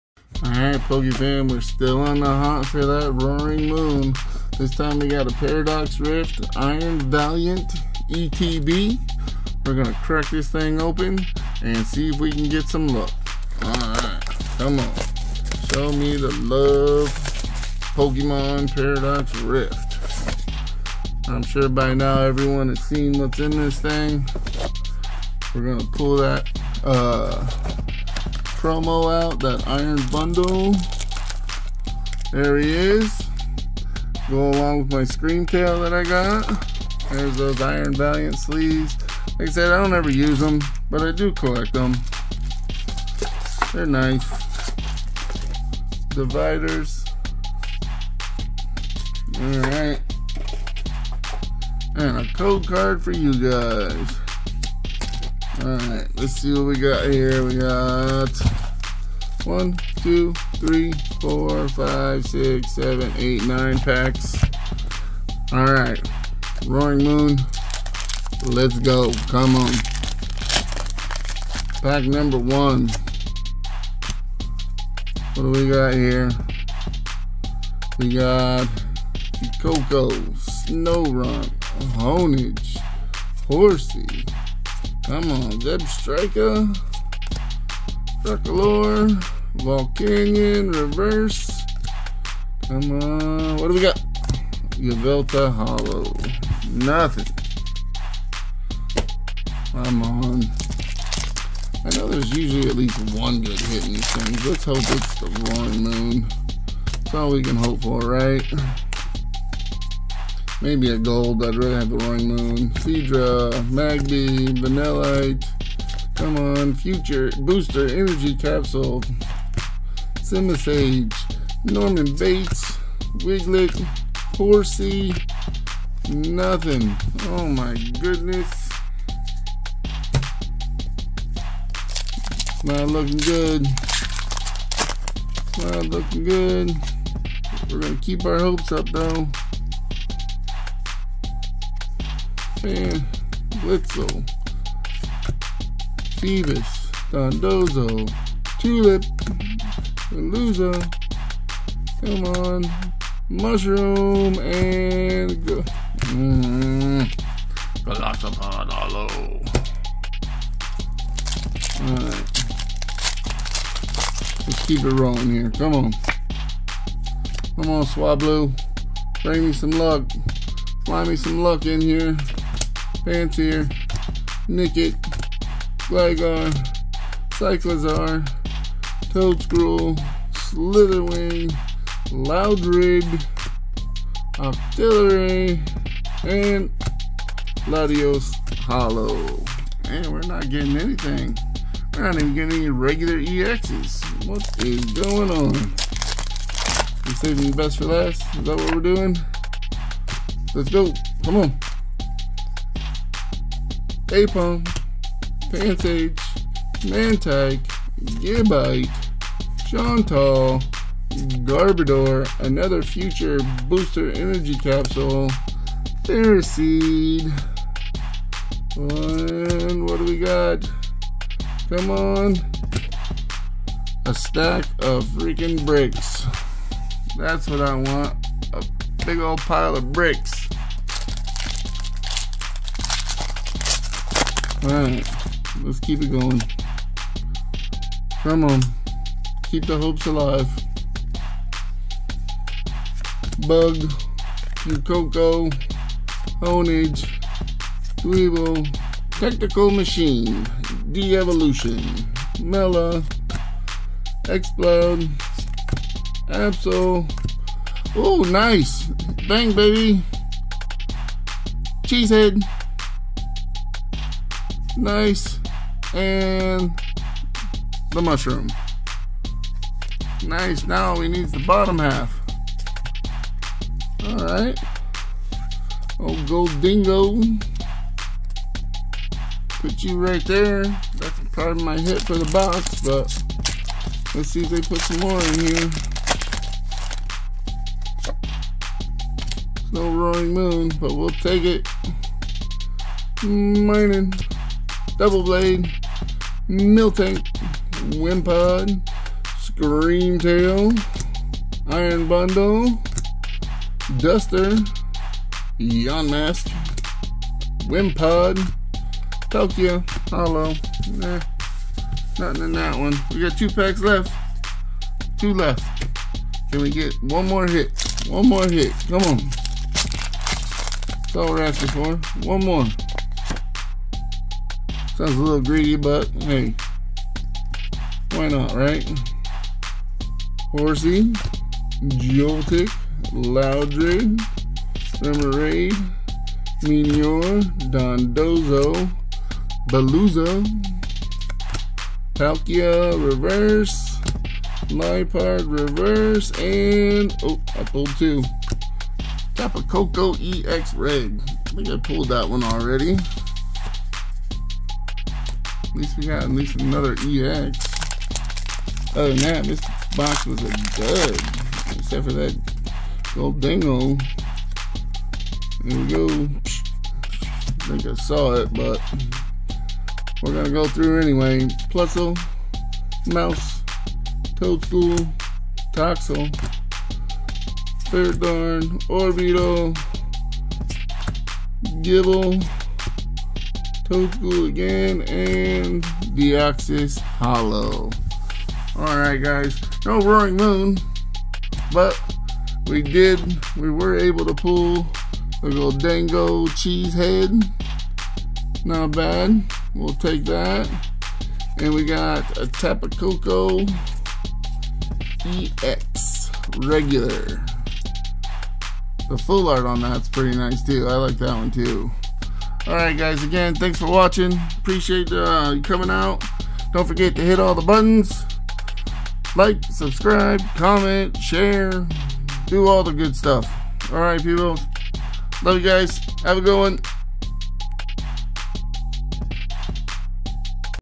Paradox Rift ETB Iron Valiant Edition
Opening the Iron Valiant ETB. Looking for more SIR's